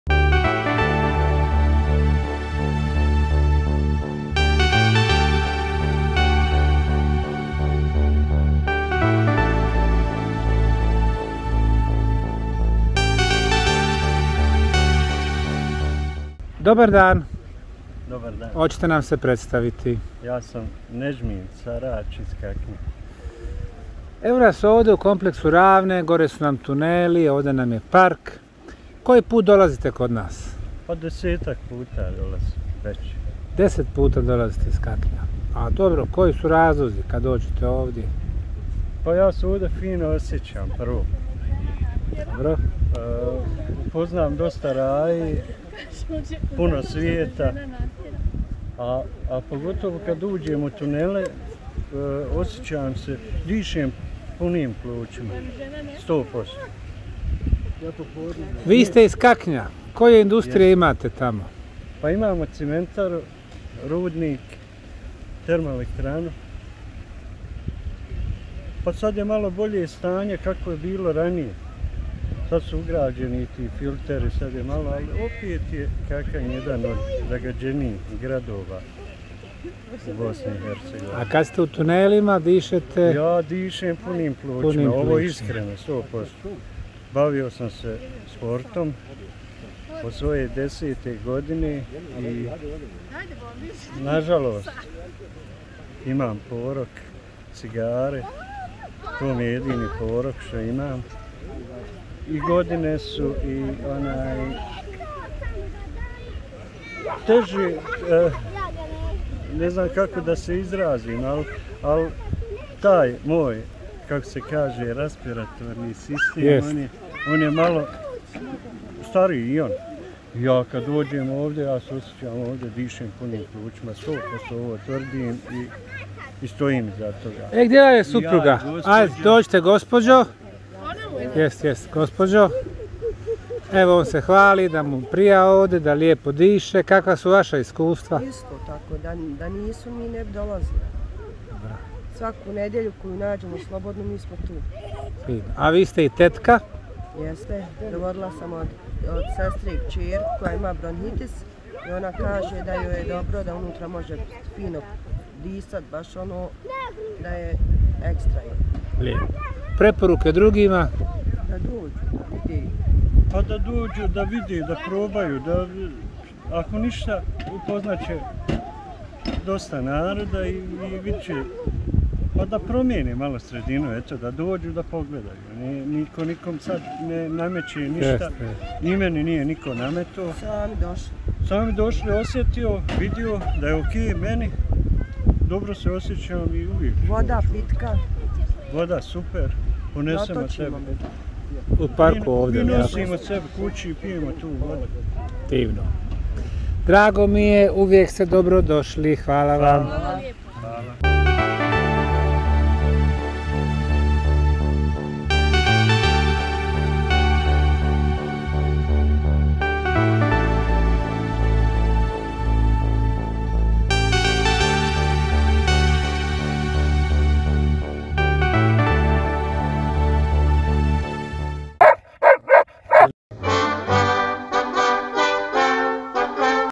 'Dišem punim plućima'
Ljekovitost tunela: Zašto Nedžmin Sarač iz Kaknja dolazi po deseti put u tunele i park u kompleksu Ravne?
Napomena: Fondacija se ne bavi liječenjem niti daje zdravstvene garancije.

Kamera: Semir Osmanagić
Montaža i muzika: Adnan Pašić